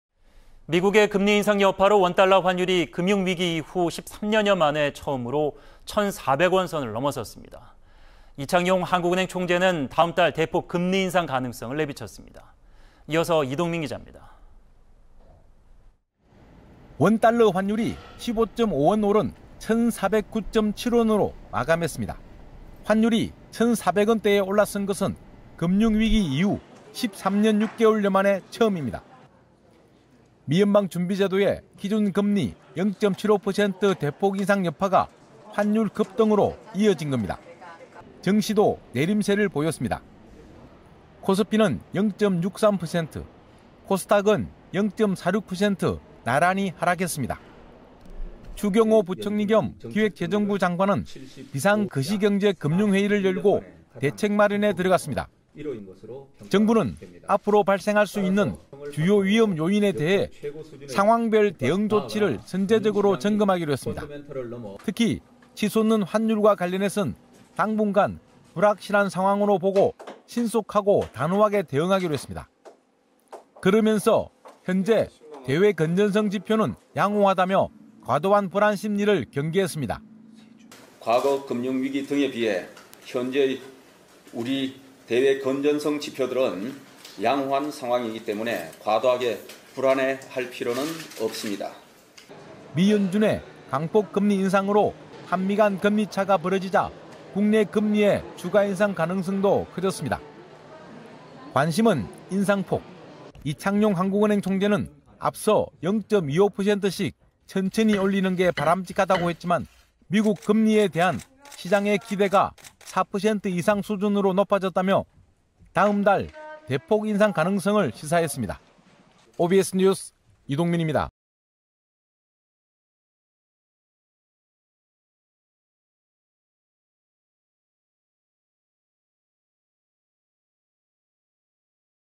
결국 환율 1,400원선 돌파... 한은 총재 "대폭 금리 인상 가능성 있다"
미국의 금리 인상 여파로
원/달러 환율이
금융위기 이후 13년여 만에
처음으로
1천400원선을 넘어섰습니다.
이창용 한국은행 총재는
다음 달 대폭 금리 인상
가능성을 내비쳤습니다.
이어서, 이동민 기자입니다.
뉴스원입니다.